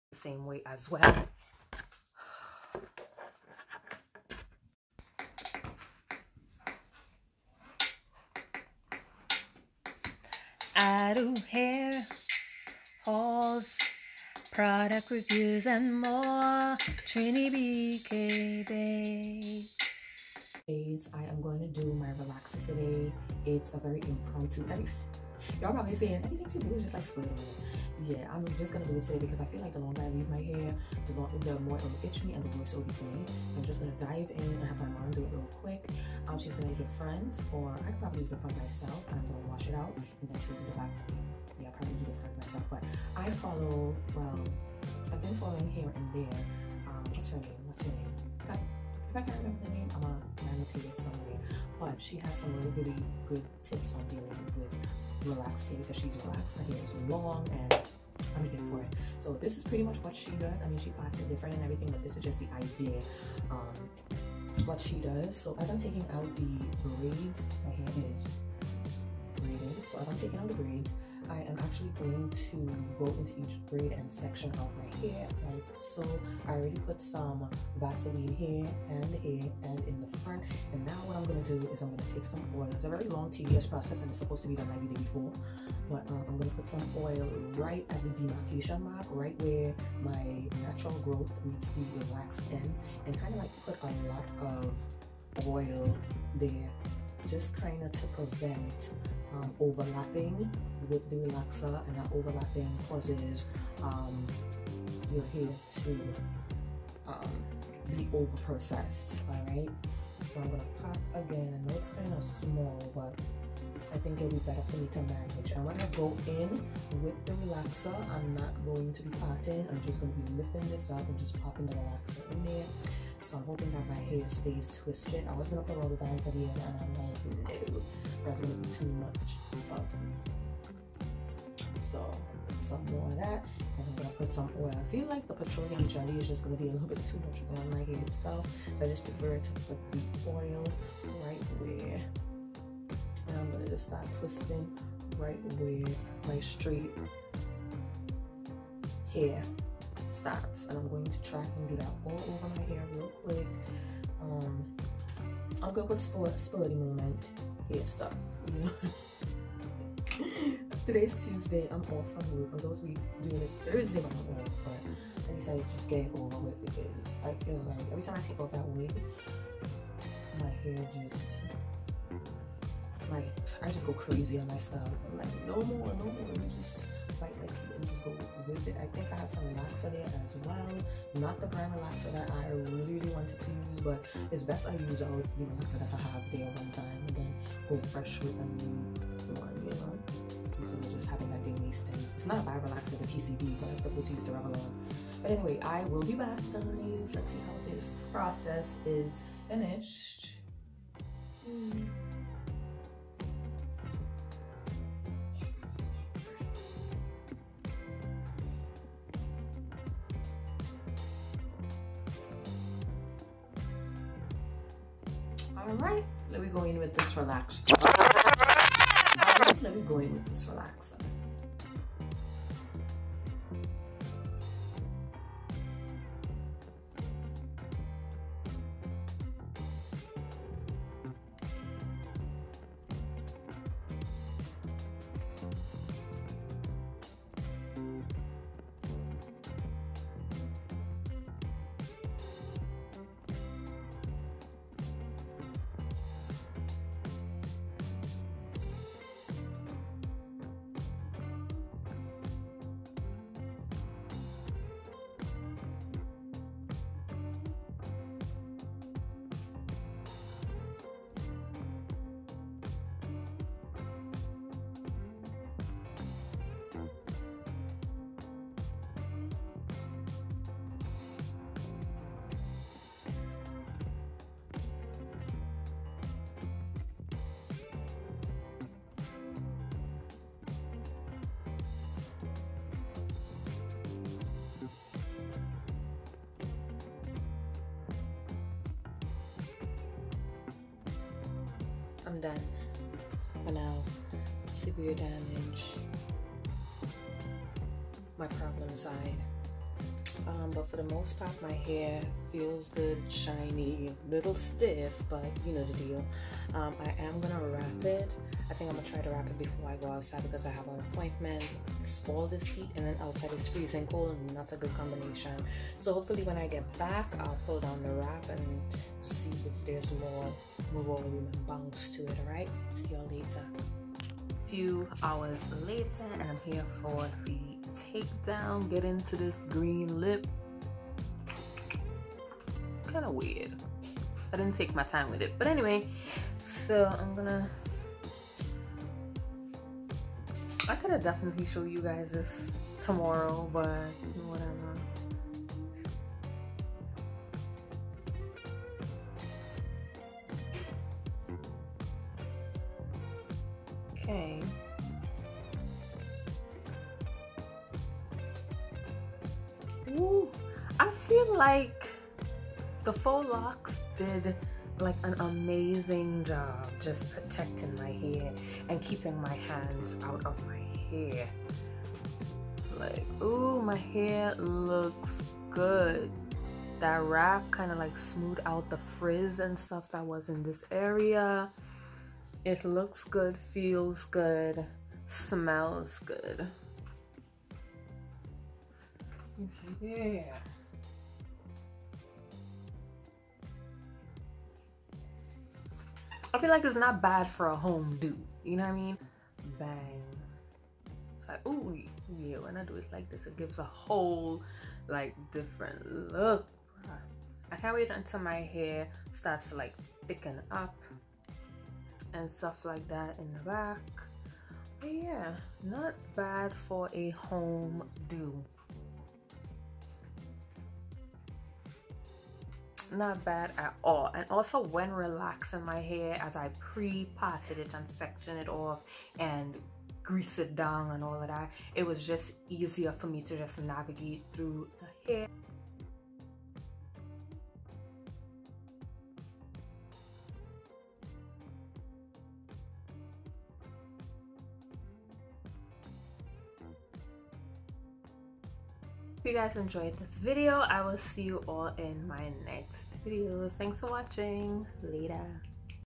Relaxer Day!! Prep & Style Pt.1
Jays, finally I got a relaxer. It's bin a while!

I did the front and my mom handled the back. It came out pretty good.

-I get a relaxer every 8-10 weeks
-If I stretch my relaxer it's usually not for more than 14 weeks
-I did not deep condition this time
-part 2 of the video coming tomorrow
-My hair was wrapped for 5 hrs

Jen